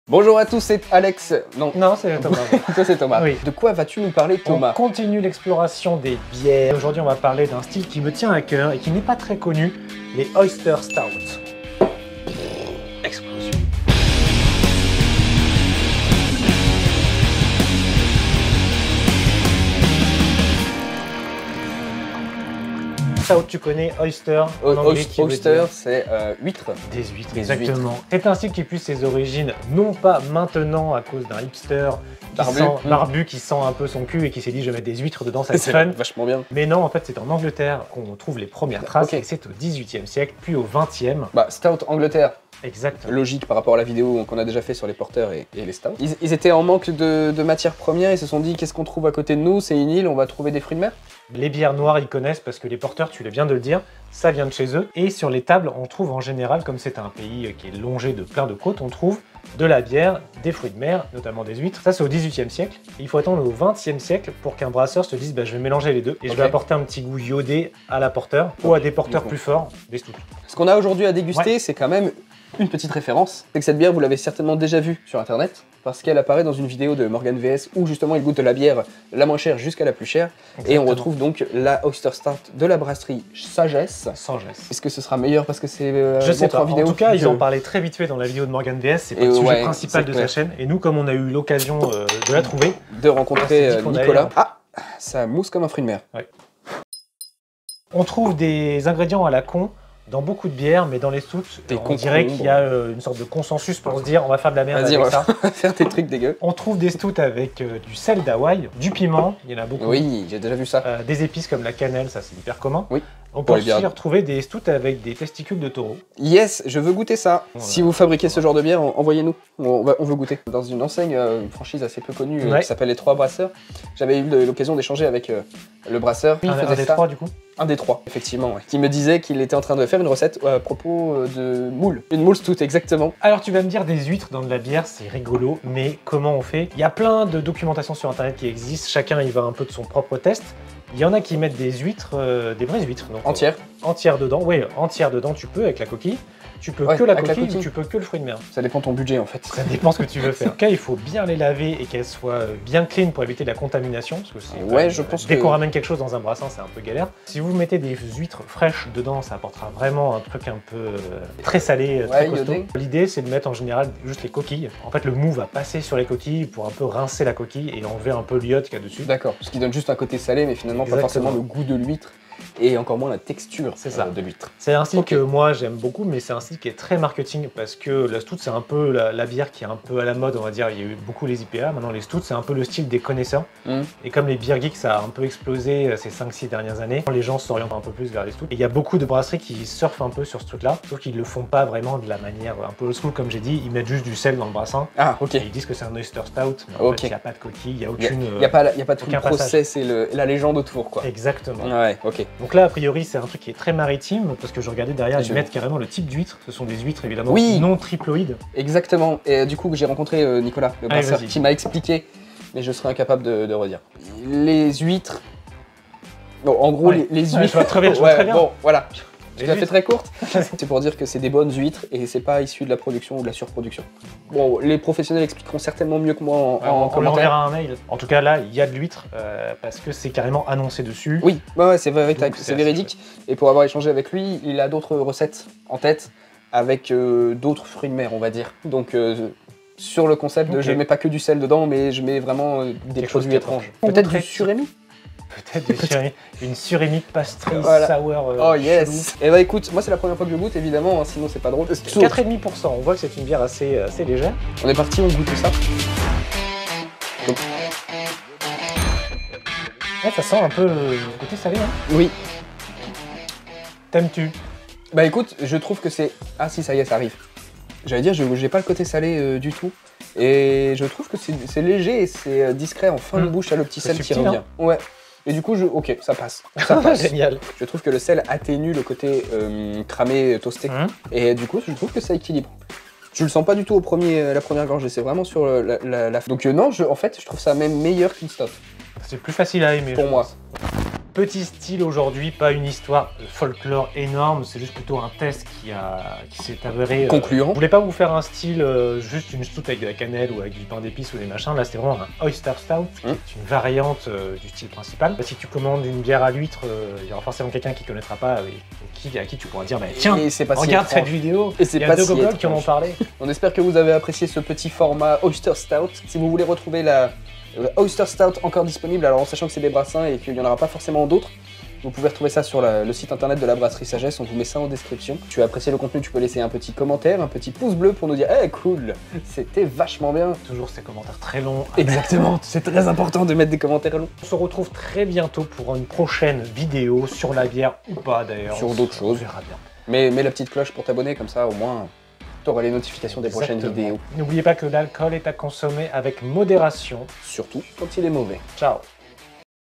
C'est quoi une bière Oyster Stout ? - Les styles de bière
Un Oyster Stout c'est quoi ? Thomas nous fait découvrir un style de bière Anglais ! Fruits de mer et bière c'est compatible et pas que en accord mets bière. Alors d'où ça vient l'Oyster Stout, depuis quand ça existe les bières Oyster Stout,  comment fabrique-t-on la bières de type Oyster Stout ? Et surtout qu'est ce que ça goûte ? On vous dit tout.

Retrouver l'Oyster Stout qu'on a goûté :
► BRASSERIE SAGESSE -

L’ abus d’alcool est dangereux pour la santé, à consommer avec modération. Surtout quand il est mauvais.